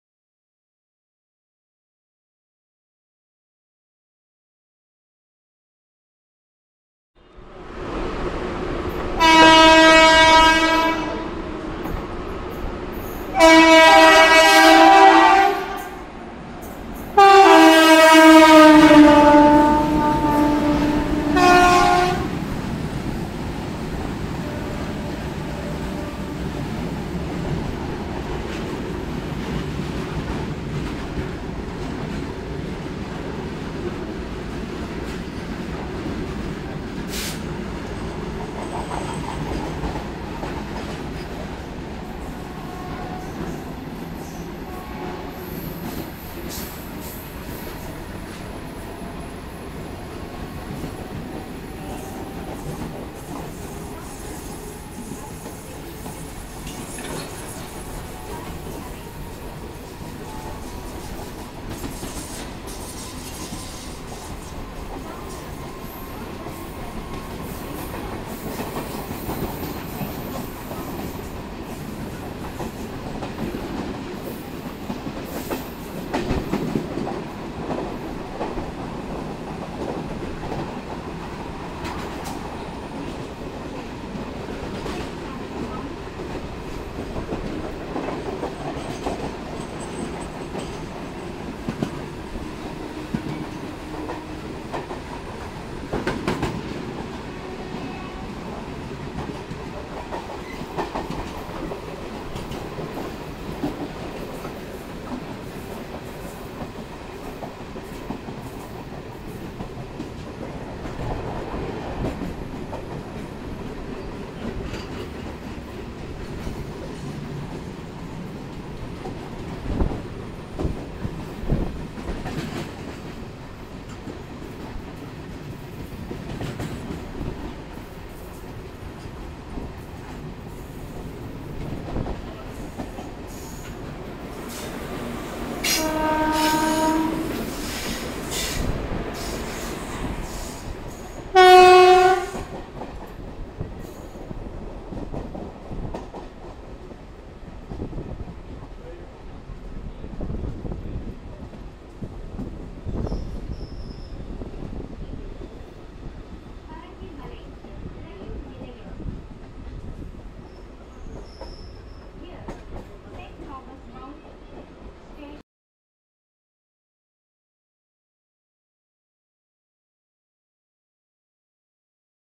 Overtaken and a re overtake !!
While I was travelling from Chennai egmore to Tambaram,during my railfanning trip I came across a loco painted brown and it seemed to look like a Wag5 and yes it finnaly was! Twin wag 5 hauling goods overtaked our emu ..After a while our emu overtook twin wag5s and you can see the lp Showing their hand!!